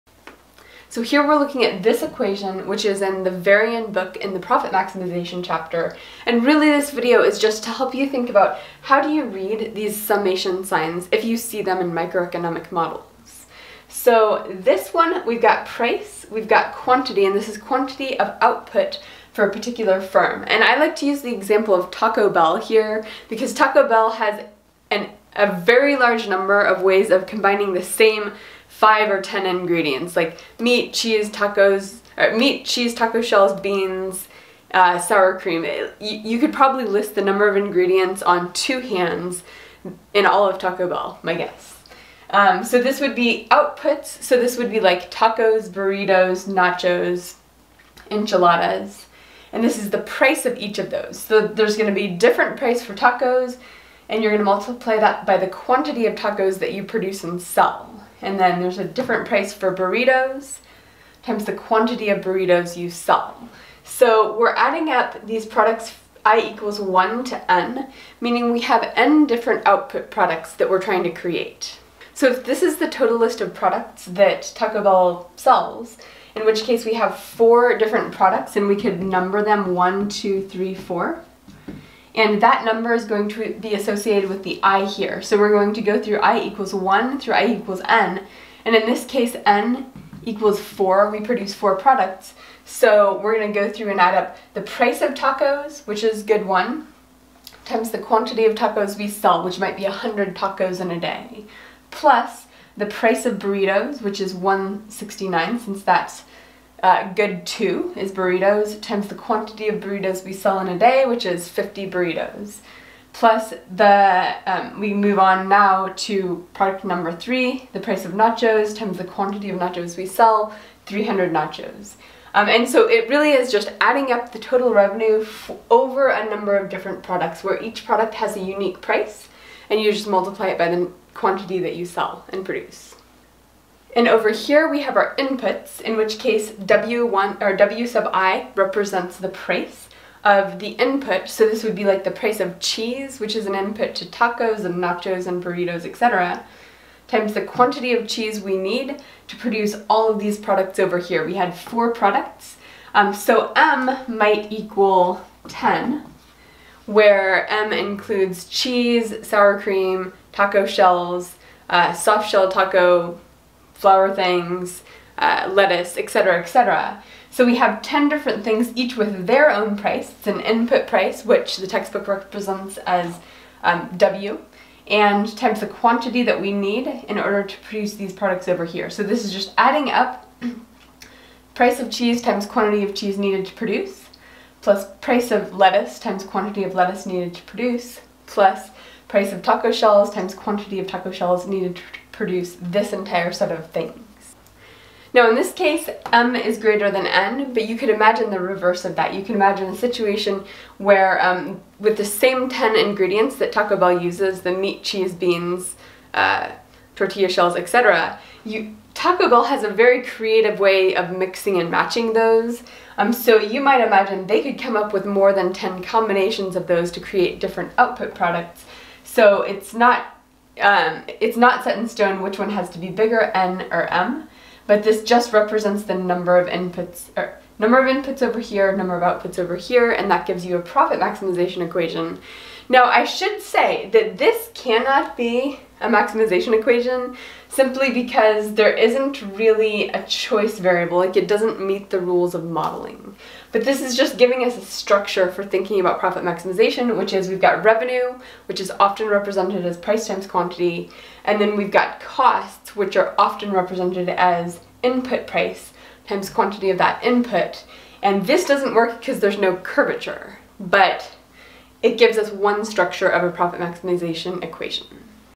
Summation Profit Max (Varian Profit Max Chapter)
Econ 262: Intermediate Microeconomic Theory
This video explains how summations work, going over the profit maximization equation near the beginning of Hal Varian's Profit Maximization Chapter.